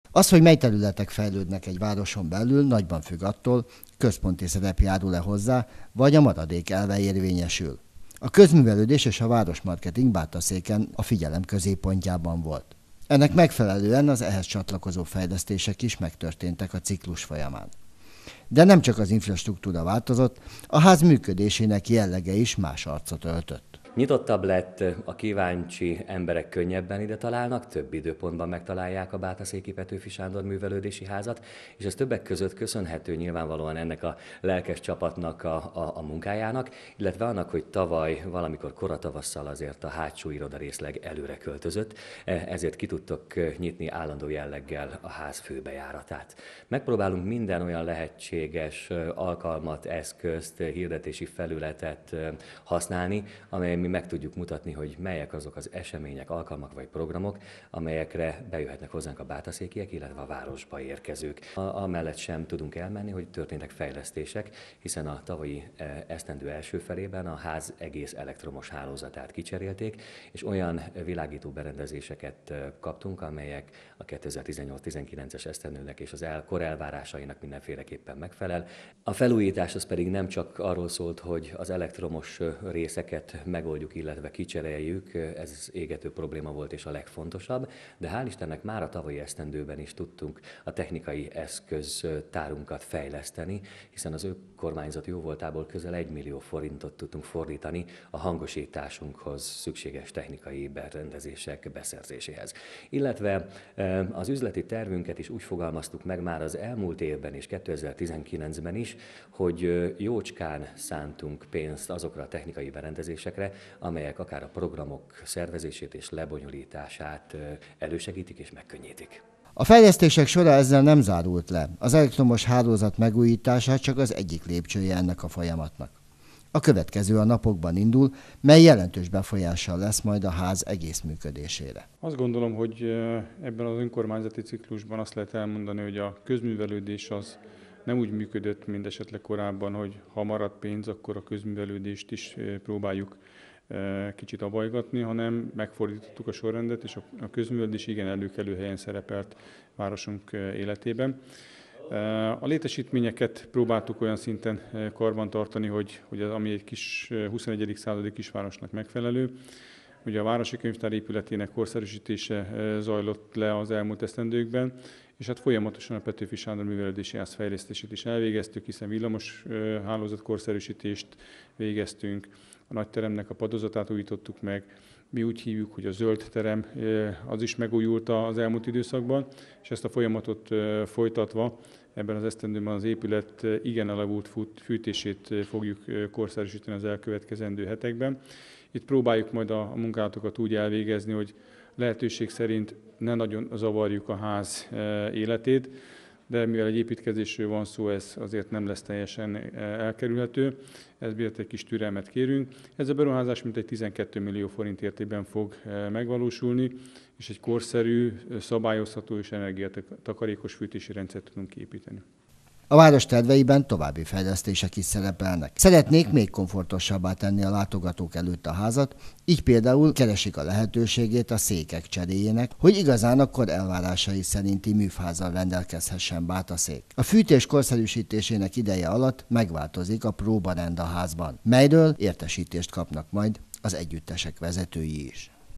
Petőfi Sándor Művelődési Ház - felújítás 20190930
A próbák vagy előadások hangjai helyett Ismét a munkazaj lesz hallható a Petőfi Sándor Művelődési Házból. A tavalyi elektromos rendszer megújítását követően ezúttal az elavult fűtésrendszer cseréjét határozta el a fenntartó önkormányzat. A munkálatok napokon belűl elkezdődnek.